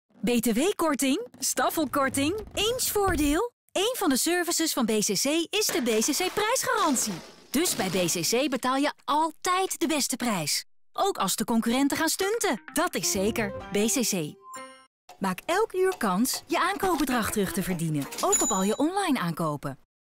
BCC Prijsgarantie - Win je aankoopbedrag terug!
Maak kans om  van 27 januari 12.00 uur tot met 31 januari 0.00 uur elk uur je aankoopbedrag terug te winnen!